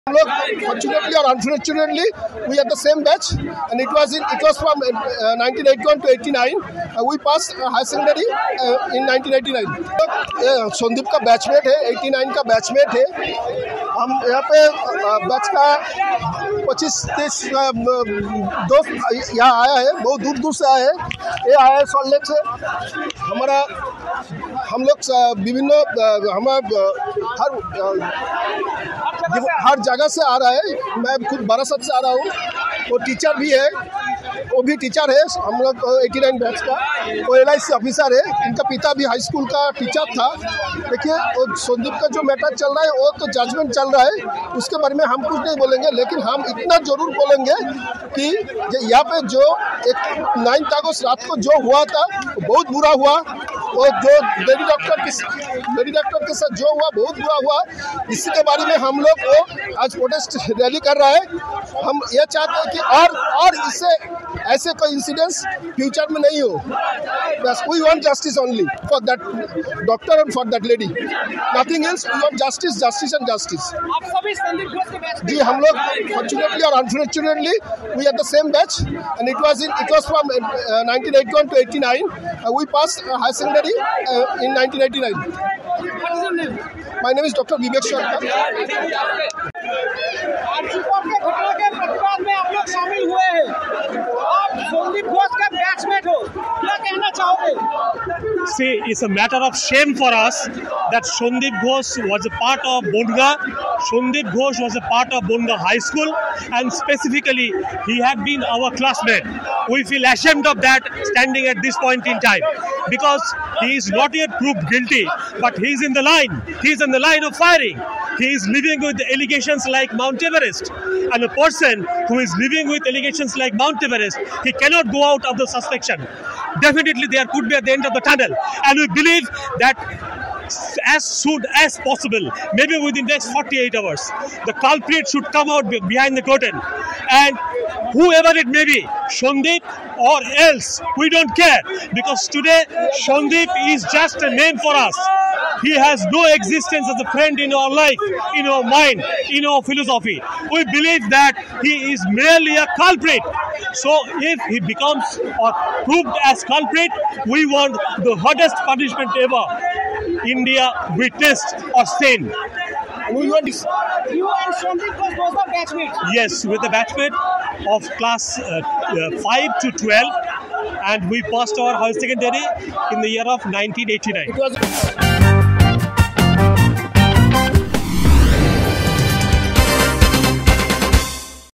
‘ଆମେ ସନ୍ଦୀପ ଘୋଷଙ୍କ ବ୍ୟାଚମେଟ’ | Odisha Reporter
କୋଲକାତା ଦୁଷ୍କର୍ମ ଓ ହତ୍ୟା ଘଟଣା। ପ୍ରତିବାଦକୁ ଓହ୍ଲାଇଲେ ସନ୍ଦୀପ ଘୋଷଙ୍କ ହାଇସ୍କୁଲ ବ୍ୟାଚମେଟ। କହିଲେ ଆମକୁ ନ୍ୟାୟ ଦିଅ। ଅଭିଯୁକ୍ତ ଯିଏ, ତାକୁ କଠୋର ଦଣ୍ଡ ଦିଅ। ଶୁଣନ୍ତୁ ପ୍ରତିକ୍ରିୟା‍...